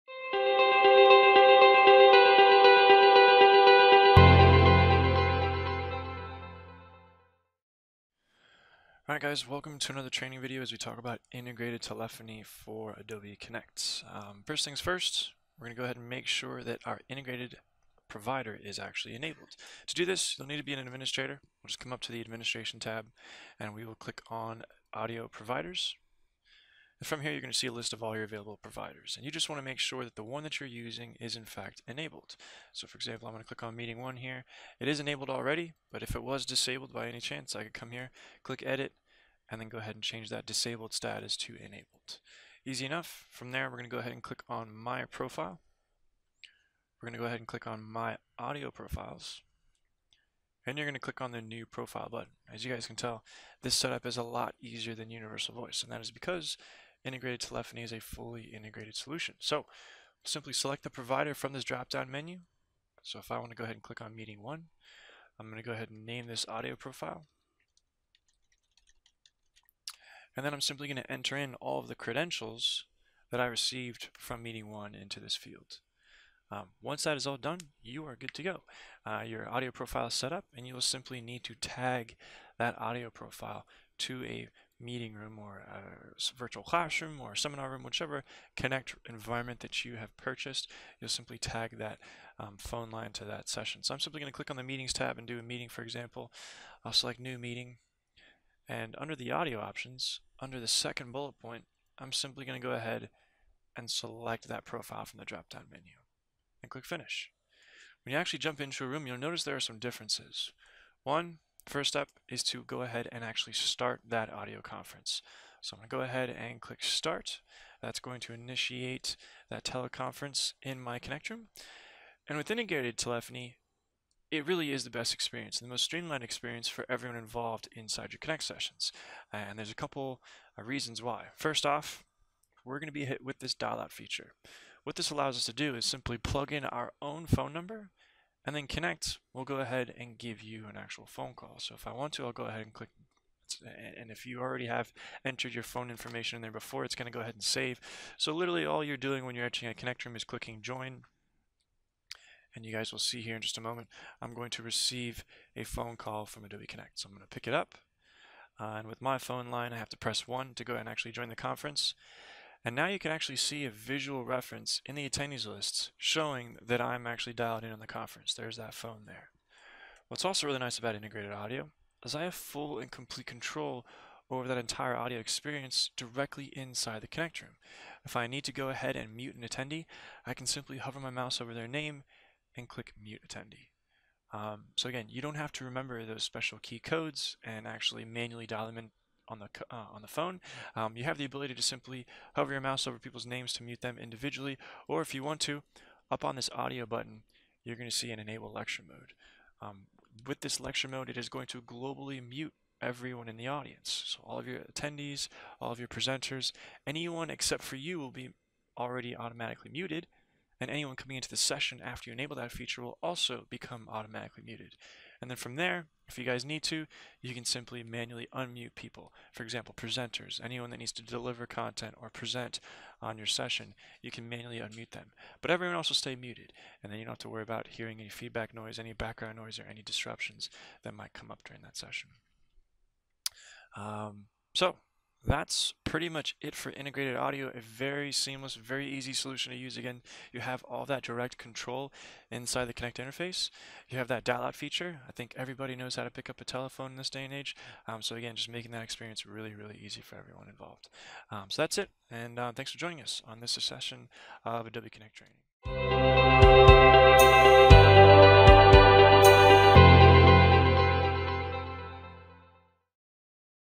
Setting Up Integrated Telephony
We will show you how easy integrated audio set up is and some of the  features that come with it!